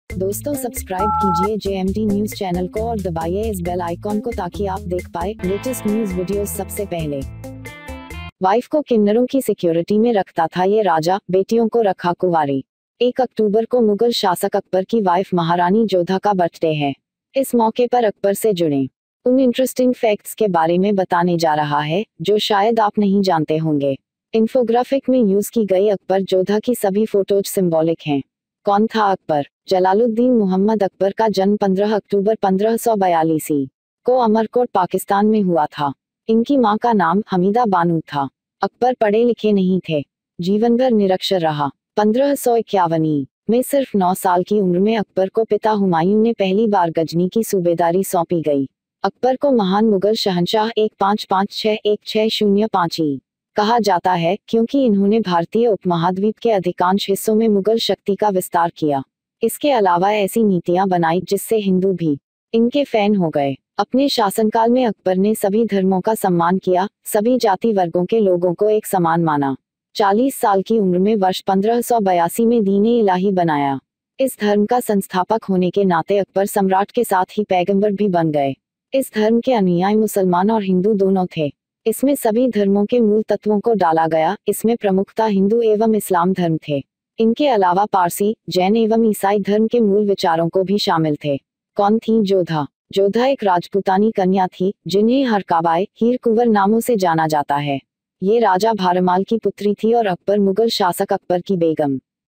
Wife को किन्नरों की सिक्युरिटी में रखता था यह राजा,बेटियों को रखा कुंवारी || Jmd News Updates
Wife को किन्नरों की सिक्युरिटी में रखता था ये राजा,बेटियों को रखा कुंवारी || Jmd News Updates ||

9 साल की उम्र में अकबर को पिता हुमायूं ने पहली बार गजनी की सूबेदारी सौंपी थी।आपको बता रहा है अकबर से जुड़े कुछ इंटरेस्टिंग फैक्‍ट्स।
कौन था अकबर |
जलालुद्दीन मुहम्मद अकबर का जन्‍म 15 अक्टूबर, 1542 ई. को अमरकोट (पाकिस्‍तान) में हुआ था। इनकी मां का नाम 'हमीदा बानू' था।अकबर पढ़े-लिखे नहीं थे। 1551 ई. में सिर्फ 9 साल की उम्र में अकबर को पिता हुमायूं ने पहली बार गजनी की सूबेदारी सौंपी गई।अकबर को महान मुगल शहंशाह (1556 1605 ई.) कहा जाता है, क्‍योंकि इन्‍होंने भारतीय उपमहाद्वीप के अधिकांश हिस्सों में मुगल शक्ति का विस्तार किया।अपने शासनकाल में अकबर ने सभी धर्मों का सम्मान किया, सभी जाति-वर्गों के लोगों को एक समान माना। 40 साल की उम्र में वर्ष 1582 में दीन-ए-इलाही बनाया। इस धर्म का संस्‍थापक होने के नाते अकबर सम्राट के साथ ही पैगंबर भी बन गए।